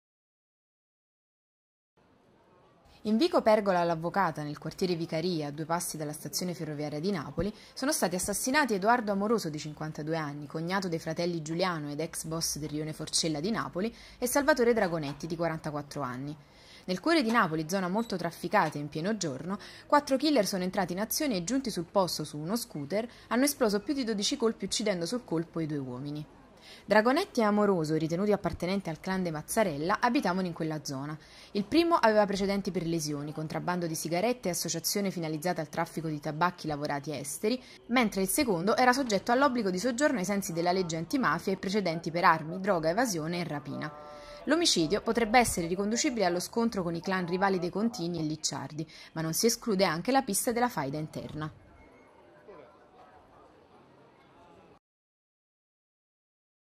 Napoli - Agguato nel centro storico, due morti (07.09.17)
- Due morti in un agguato compiuto oggi pomeriggio a Napoli, nel quartiere Vicaria, in vico Pergola all’Avvocata. Le vittime sono Edoardo Amoruso e Salvatore Dragonetti.

Paura tra residenti e persone che si trovavano in zona durante la sparatoria. Ad agire, secondo una prima ricostruzione, diversi sicari che hanno esploso numerosi colpi di pistola. Sul posto le forze dell'ordine che hanno avviato le indagini.

Sia Dragonetti che Amoroso abitavano nel vicolo dove è scattato il raid. I due sono ritenuti appartenenti al clan dei Mazzarella e questo duplice omicidio potrebbe essere riconducibile allo scontro con i clan rivali dei Contini e Licciardi. Non si esclude anche la pista della faida interna.

Dragonetti era imparentato ai fratelli Giuliano, ex boss del Rione Forcella, avendo sposato una nipote. Dragonetti aveva precedenti per lesioni e associazione a delinquere per contrabbando di sigarette. Più pesanti i precedenti di Amoroso, che era sottoposto a un obbligo di soggiorno ai sensi della legge antimafia; aveva anche precedenti per reati legati all’uso delle armi, allo spaccio di droga, evasione e rapina. (07.09.17)